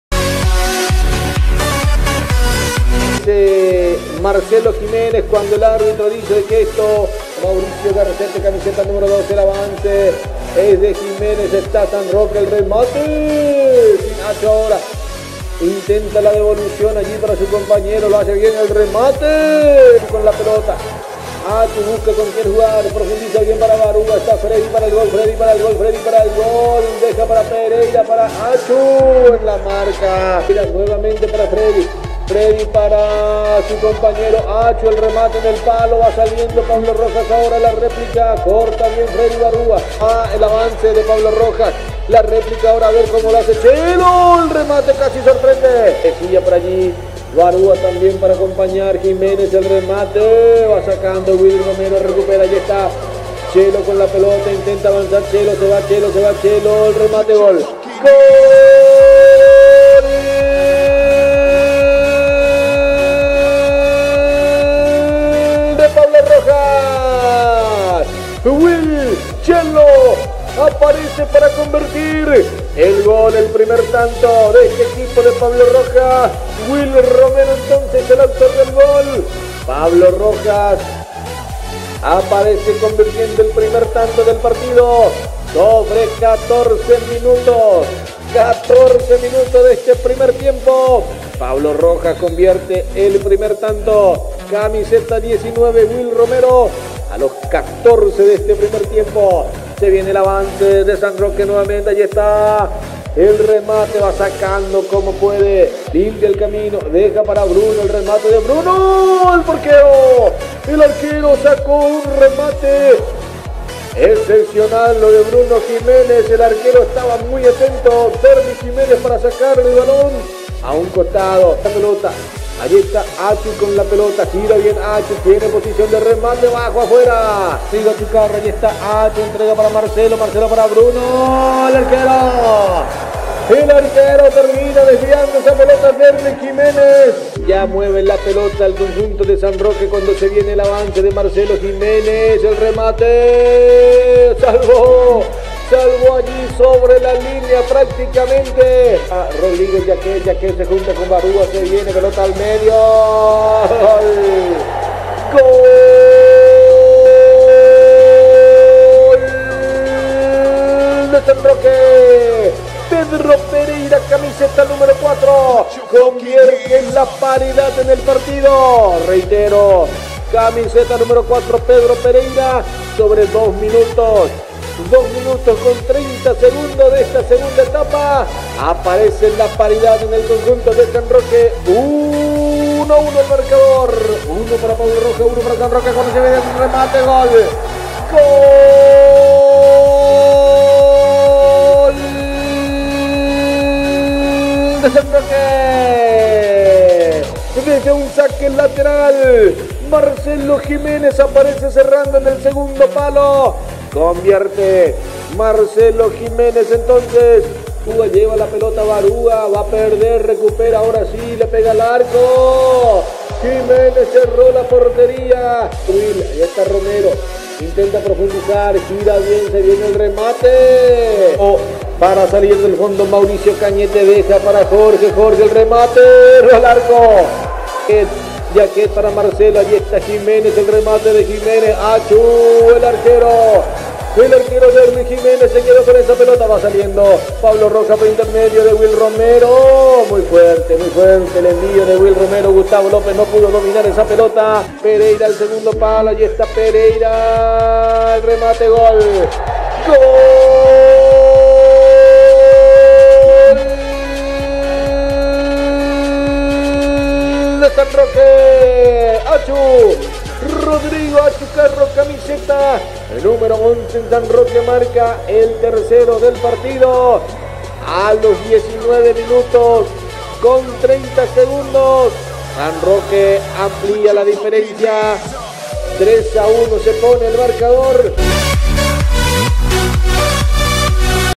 SAN RQOUE 3 1 PABLO ROJAS F1 GA
COPA INTERCOMERCIAL DE CLUBES
Gran debut de San Roque que vuelve al ruedo de competencias en esta primera fecha de la Copa Intercomercial de Clubes vence 3-1 a Pablo Rojas que comenzó sorprendiendo con el gol del Chelo Romero y en una gran reacción de San Roque dieron vuelta el marcador para ganar con goles de Pedro Pereira, Marcelo Giménez y Rodrigo Achucarro.